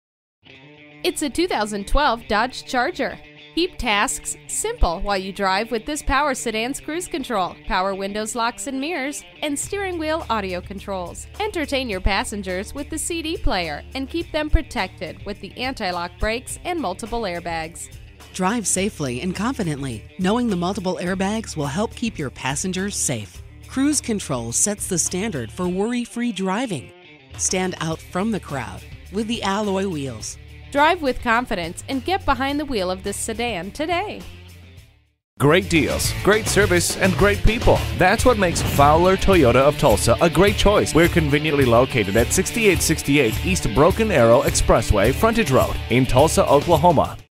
2012 Dodge Charger Tulsa, OK
Year: 2012
Make: Dodge
Model: Charger
Trim: SXT
Engine: 3.6 Liter 6 Cylinder
Transmission: 8 Speed Automatic
Color: Silver
Interior: Beige
Stock #: 130322AA

Fowler Toyota
Address:
6868 E Ba Frontage Rd